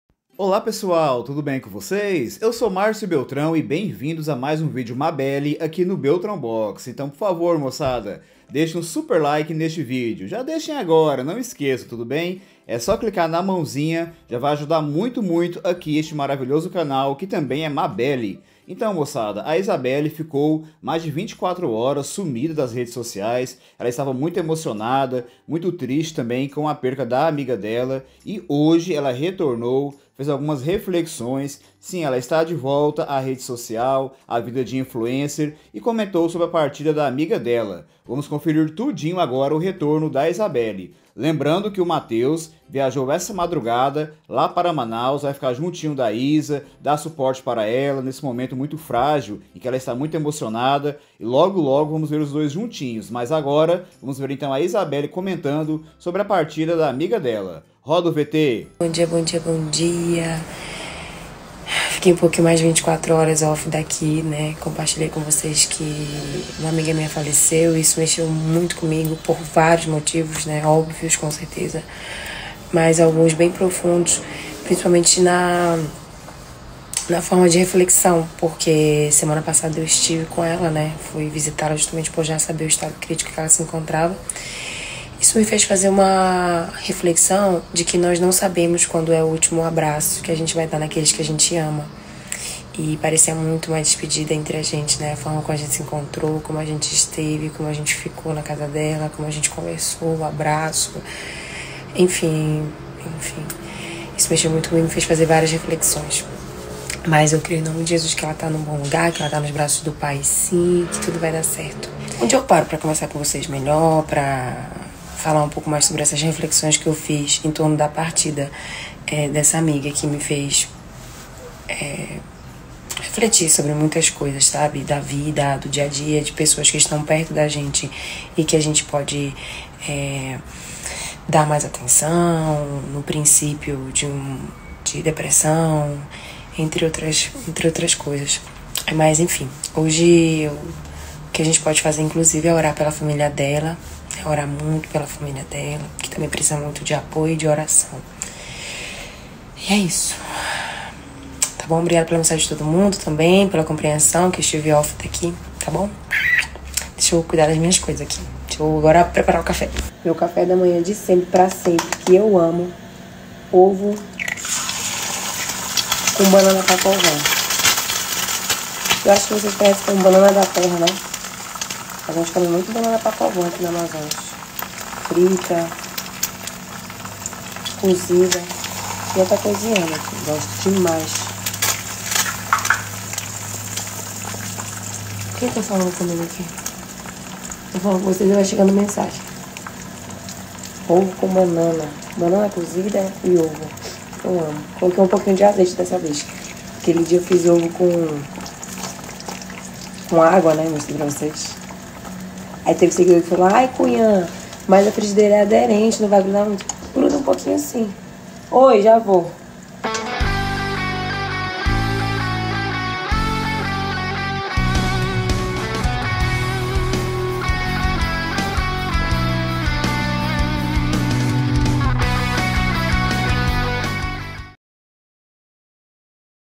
ISABELLE FALA EMOCIONADA SOBRE A SUA AMIGA QUE PARTIU!
Mabelle